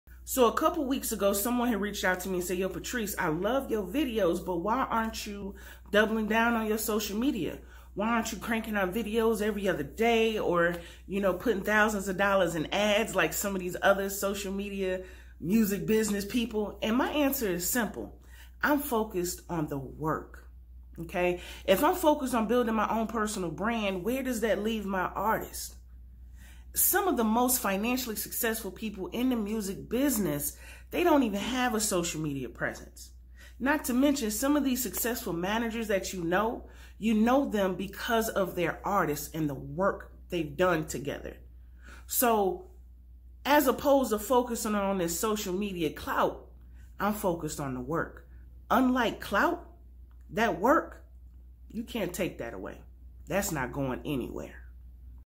Clout won't pay your bills. The work does.
Clout is a very dangerous drug. When the bubble burst, it's going to get real ugly out here. Which is one of the reasons why I don't focus on it.

It's also dumb how so many will chase those with "clout" who don't even care about you, but will look down on the people around them who are working just as hard, or harder. Not realizing that the person you're shittin on will be in that position you're lusting over sooner than you believe.

Clout will only get you so far. But the work you actually put in won't ever fade away.

*previously recorded in 2019 for Instagram*